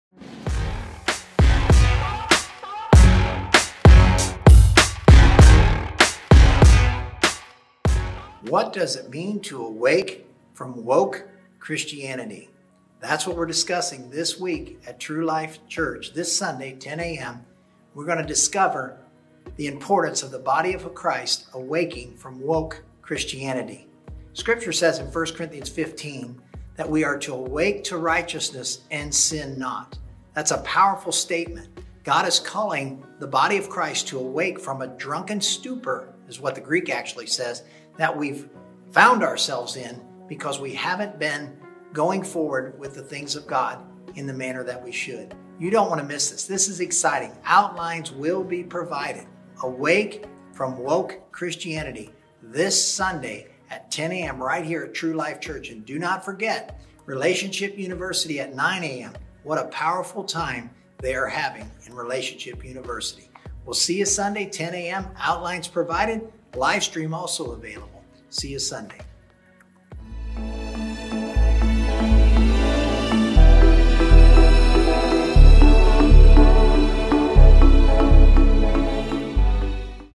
Preview Trailer - Awake from Woke Christianity
Join us this Sunday at True Life Church for a new message  titled "Awake from Woke Christianity" Livestream available.